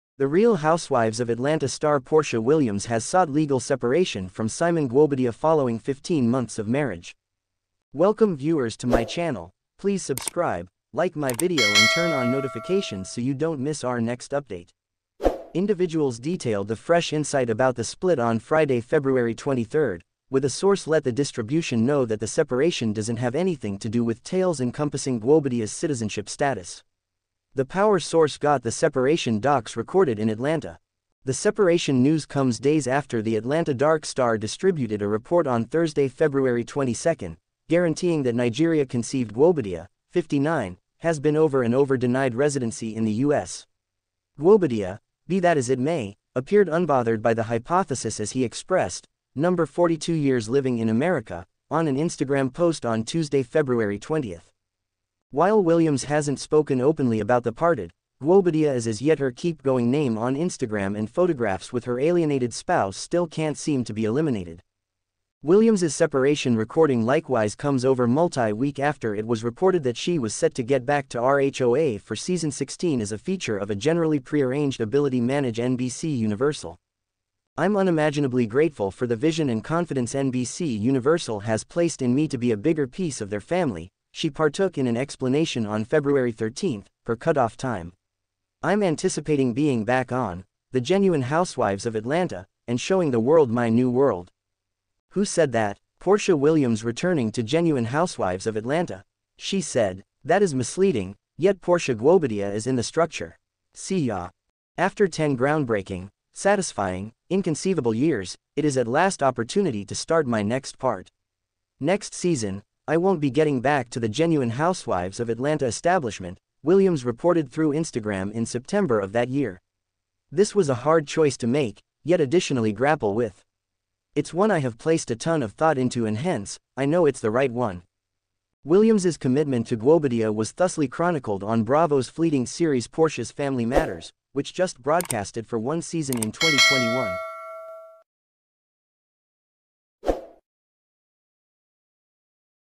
Simon shocked as porsha Williams kicked him out of his own house after divorce settlement gone wrong
The real Housewives of Atlanta star Porsha Williams has sought legal separation from Simon Guobadia following 15 months of marriage.

Individuals detailed the fresh insight about the split on Friday, February 23, with a source let the distribution know that the separation doesn't have anything to do with tales encompassing Guobadia's citizenship status. The power source got the separation docs recorded in .